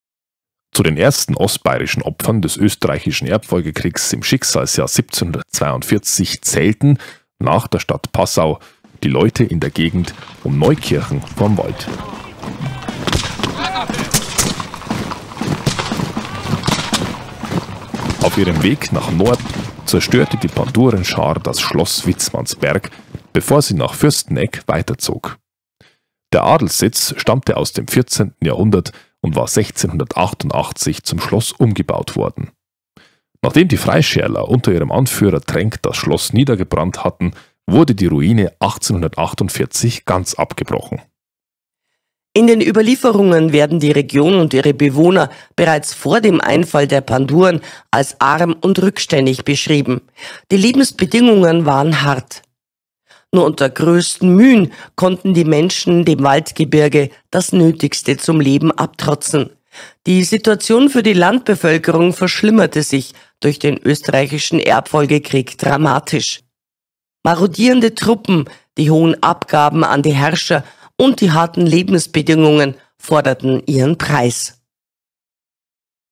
Neukirchen vorm Wald - Audioguides Pandurensteig
Zu den ersten ostbayerischen Opfern des Österreichischen Erbfolgekrieg im Schicksalsjahr 1742 zählten nach der Stadt Passau die Leute in der Gegend um Neukirchen vorm Wald...